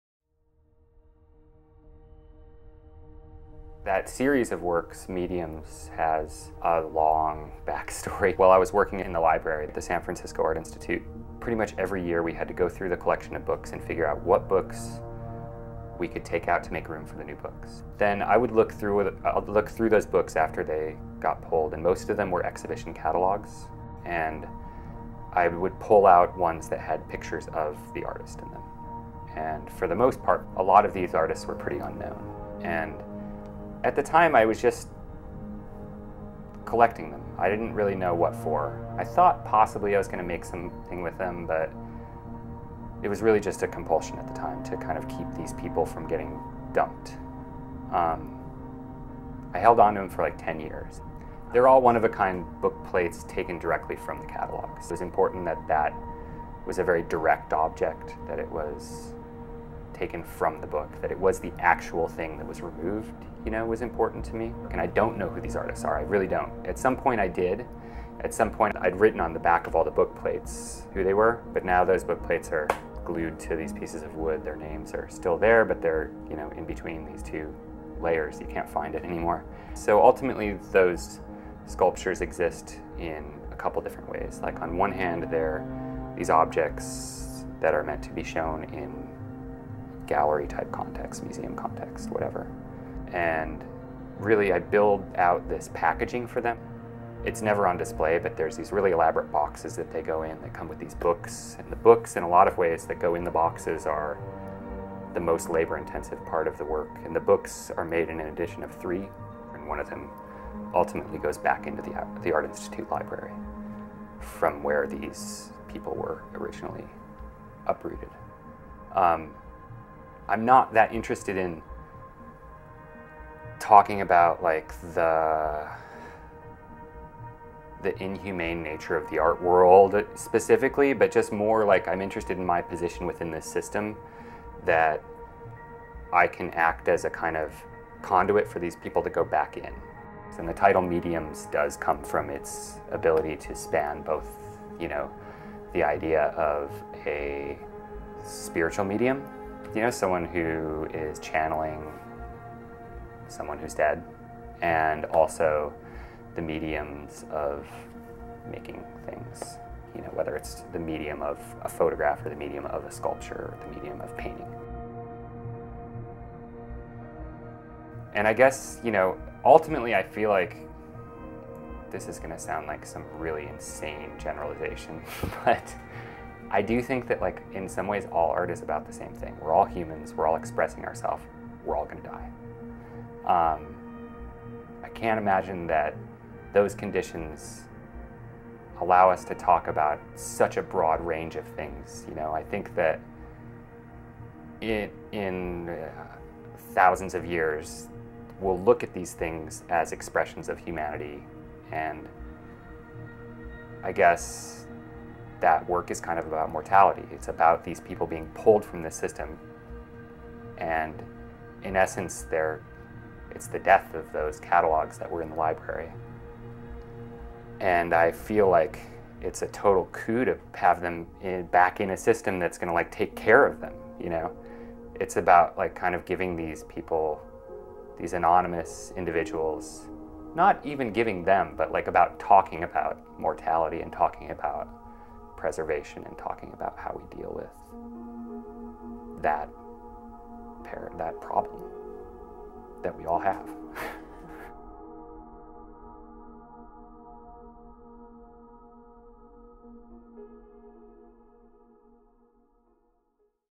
Will Rogan's Mediums channel artists lost to time
As a librarian at the San Francisco Art Institute, Will Rogan came to collect book plates from soon-to-be discarded exhibition catalogues. His series Mediums (2010-ongoing) repurposes these objects to address questions of preservation, anonymity, legacy, and mortality.